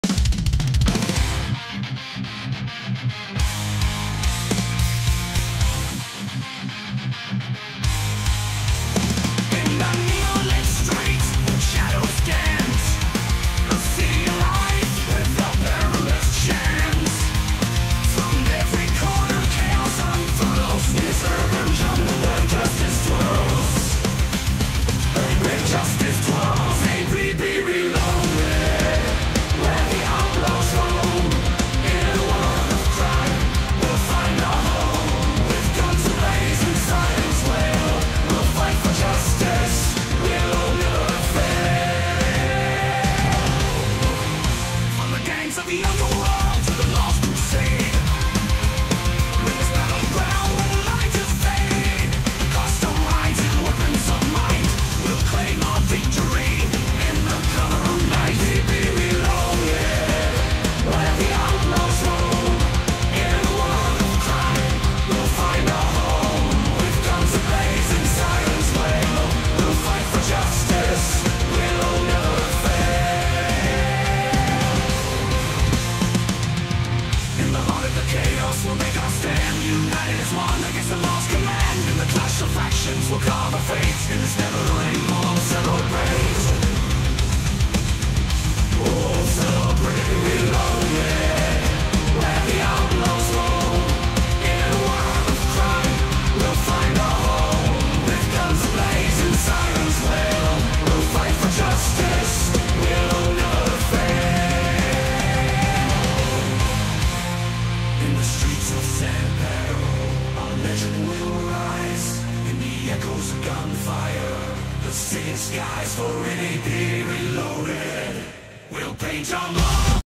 Music for APB: Reloaded....maybe?
let me know what you think of this AI Generated song for apb